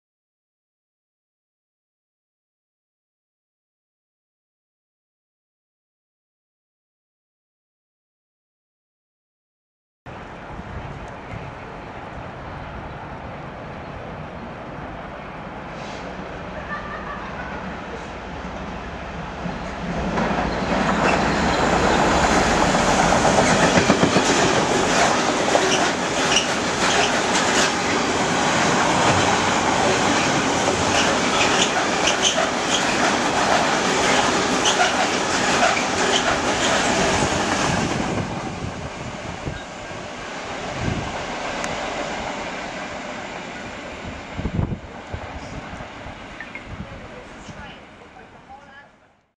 The 2nd of the 2 LUL engineers is 6Z62 Wellingborough-Barking with ex-DRS 66403 leading and 66701 on the tail. 22/01/11.